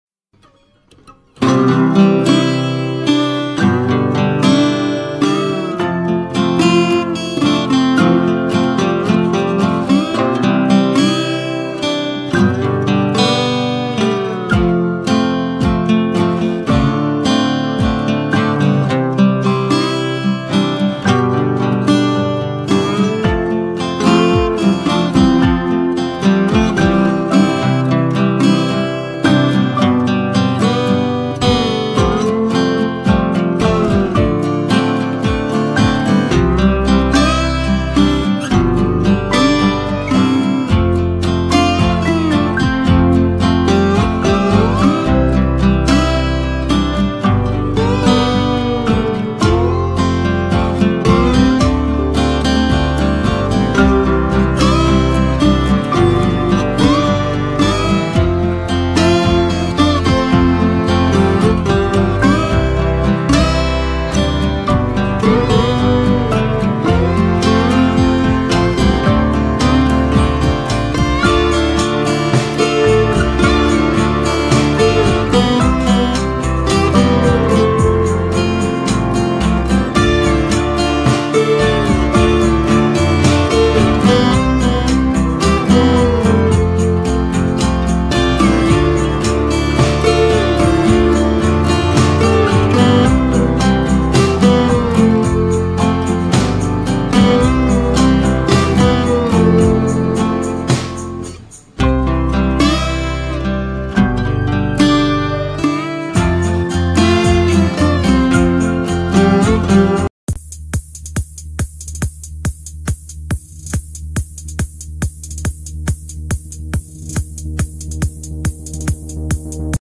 理科太太挾百萬粉撈很大 3巨星嘉賓曝光 - 自由娛樂
理科太太（左）出席記者會。（翻攝官方臉書）〔記者羅凌筠／台北報導〕網紅「理科太太」去年6月開設頻道火速竄紅，元宵節喜破百萬訂閱，代言、業配、時尚活動接不停，昨出席SONY手機新品發表，被問到價碼有沒有跟著人氣飆漲？理科太太瞟向身旁求救：「有人要幫我一下嗎？」不過聽到「百萬」等字眼，馬上敏捷的反駁「沒有」。 理科太太人紅是非多，還被網友寫文抨擊「五個理由告訴你為什麼理科太太是個智障」，談到這些非議，...